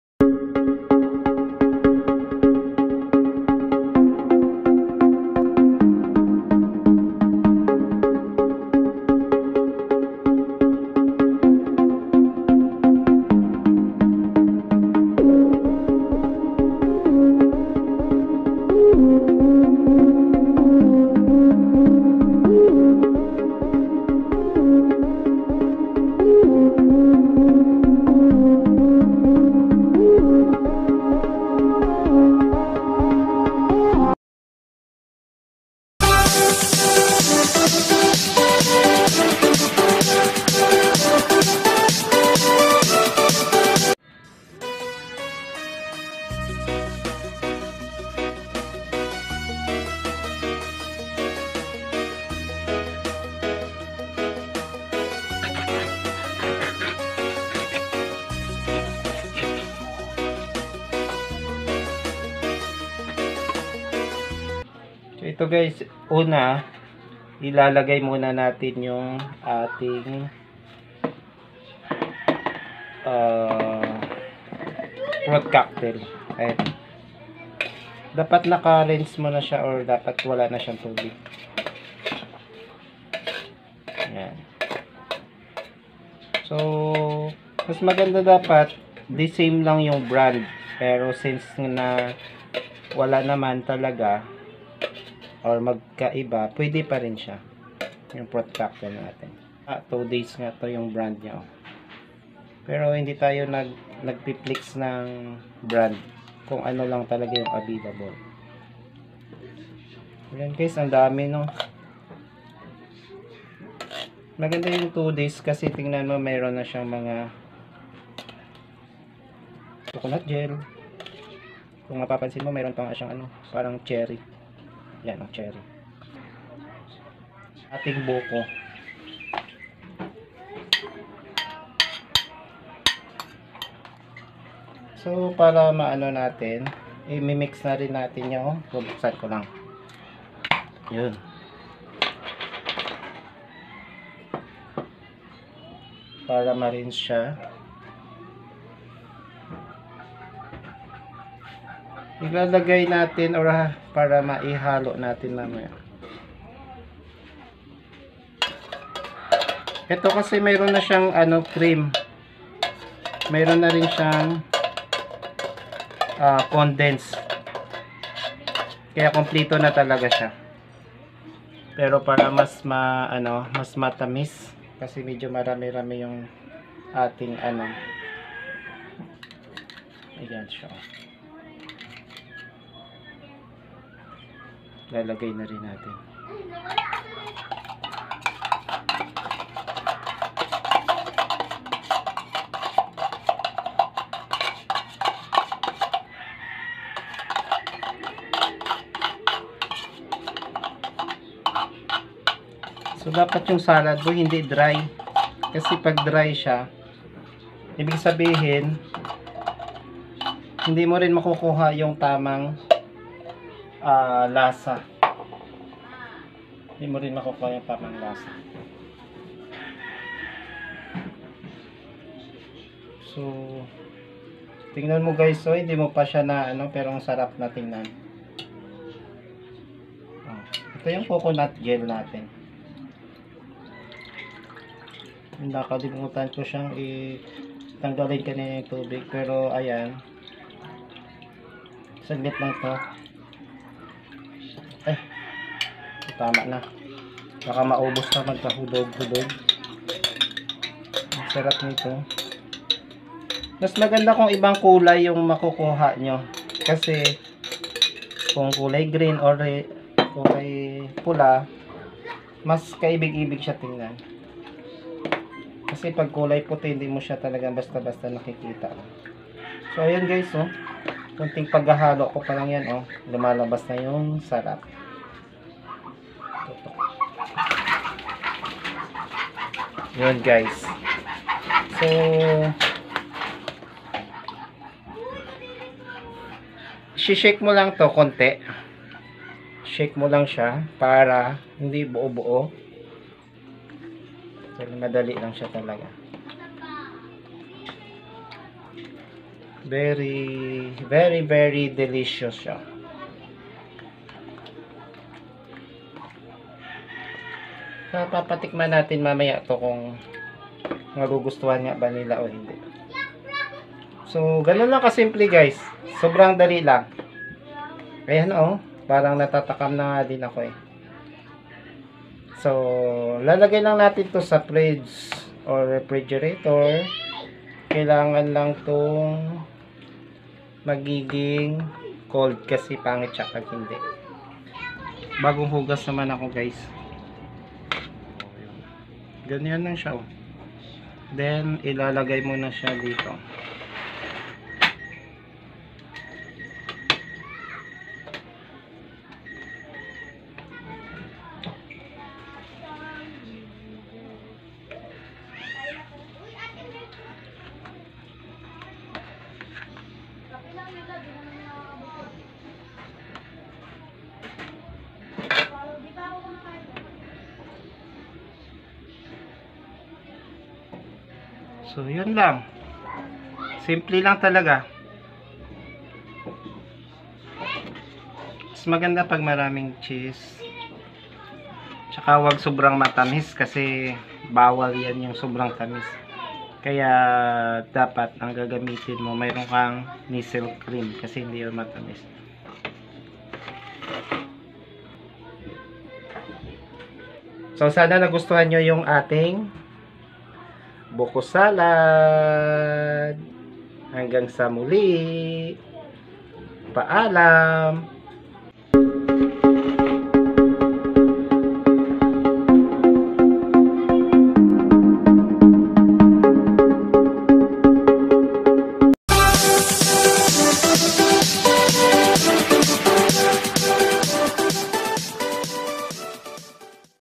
Buko Salad
Ingredients:
2 cans 836gms fruit cocktail
2  Buko (coconut young)
1 bottle Coconut Gel
1 can condense milk
1 can creamdensada
1 Neslte cream
1 Cheese